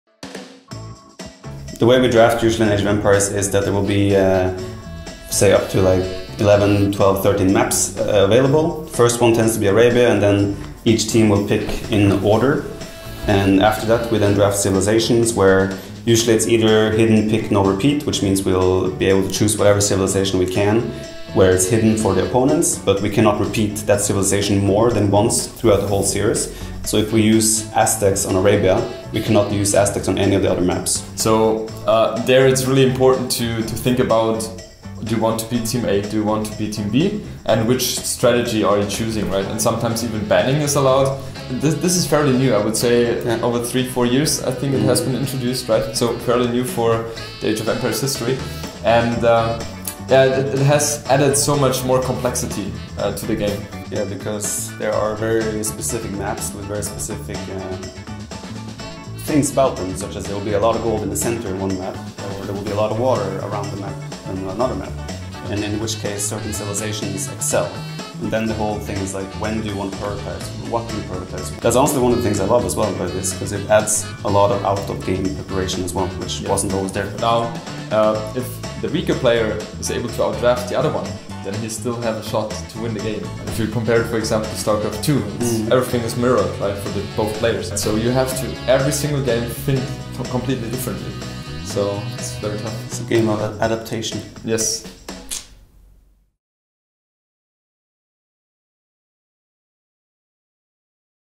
GamerLegion - THE BEST WAY TO DRAFT IN AoE (With TheViper and JorDanTV)
We asked TheViper and JorDanTV how you effectively draft in AoE.
Safe to say they are true experts in the matter!